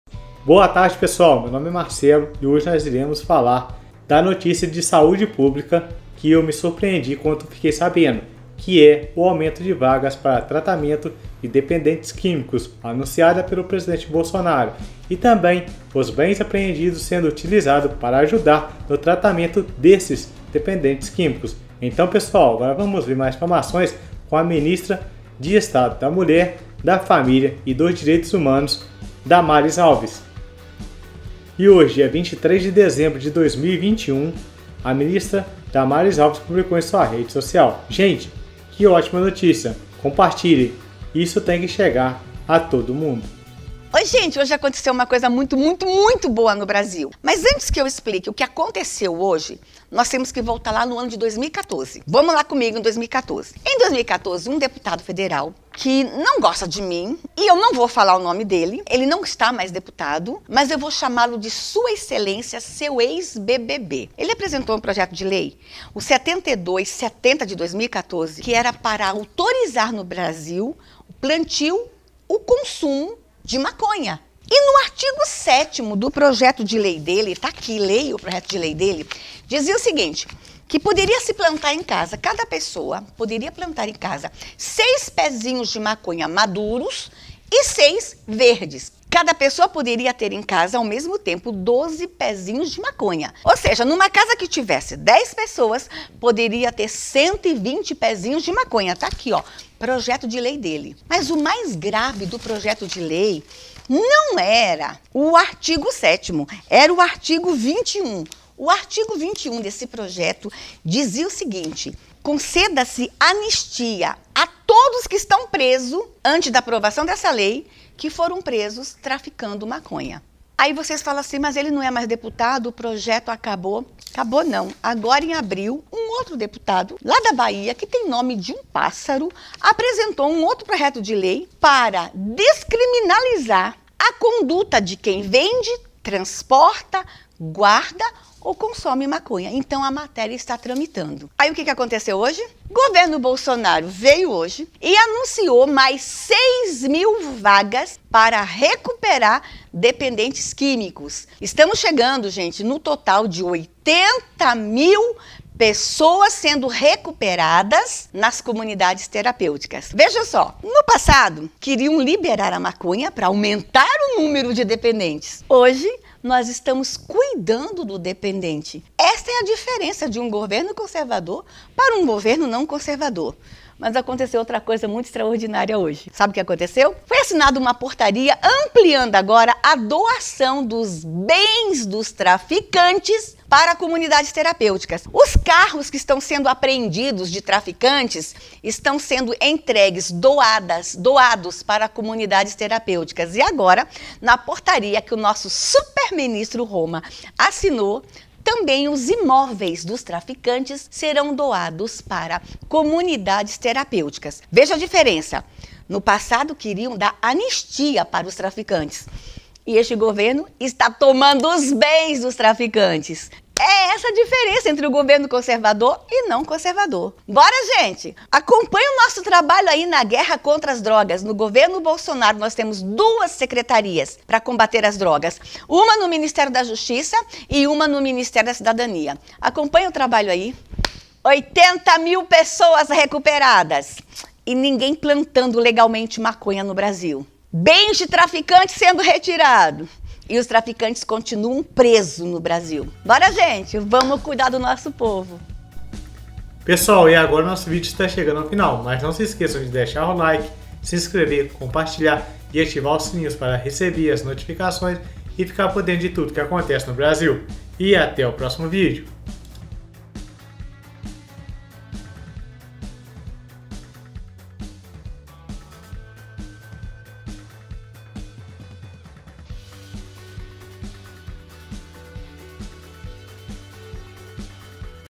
VEJA OQ BOLSONARO FEZ ! ELE É MUITO CORAJOSO !
Presidente Bolsonaro faz algo impensavel em governo passado para controlar a quantidade de dependentes quimicos no Brasil